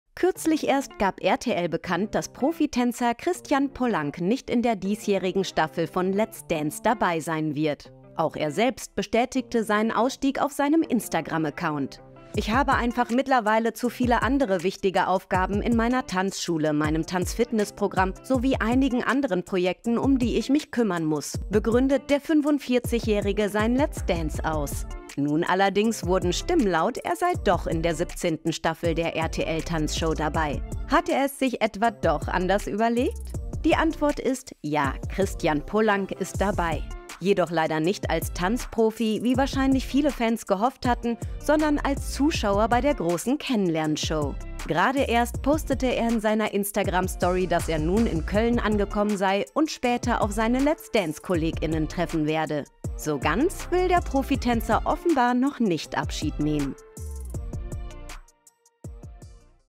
Let's Dance: Kommt Christian Polanc doch zurück?
Ist Profitänzer Christan Polanc doch in der 17. Staffel Let's Dance dabei?